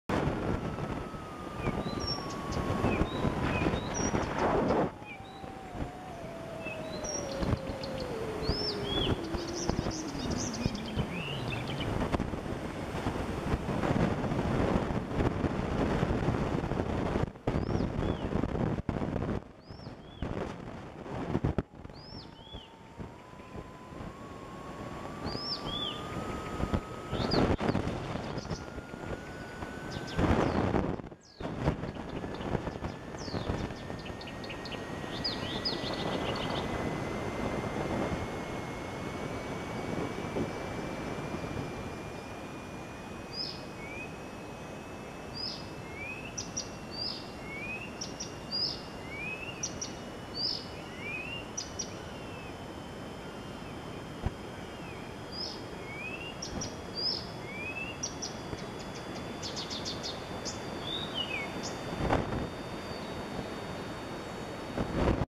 Zaroślówka (Acrocephalus dumetorum) w Parku Reagana w Gdańsku
Jedna z dwóch zaroślówek śpiewających o poranku 19 czerwca 2020 w Parku Reagana w Gdańsku